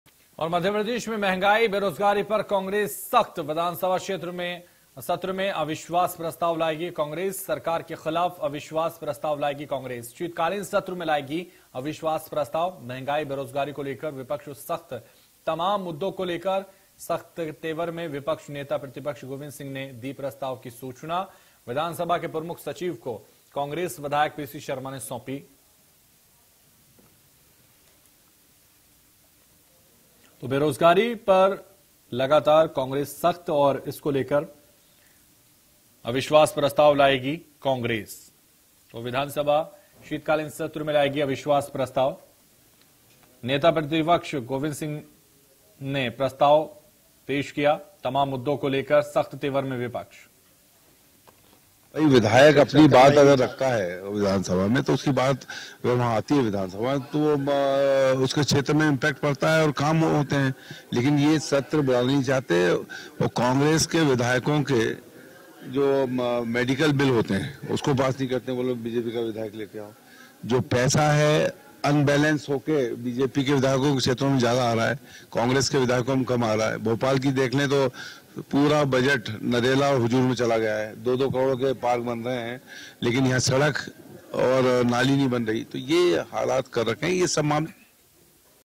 Madhya Pradesh News : महंगाई, बेरोजगारी दूर करने के लिए Congress उठाएगी सख्त कदम
महंगाई-बेरोजगारी पर कांग्रेस सख्त,विधानसभा सत्र में अविश्वास प्रस्ताव लाएगी कांग्रेस..सरकार के खिलाफ अविश्वास प्रस्ताव लाएगी कांग्रेस,विस. शीतकालीन सत्र में लाएगी अविश्वास प्रस्ताव |

Congress strict on inflation-unemployment, Congress will bring no confidence motion in the assembly session..Congress will bring no confidence motion against the government, Vis. Will bring no-confidence motion in winter session.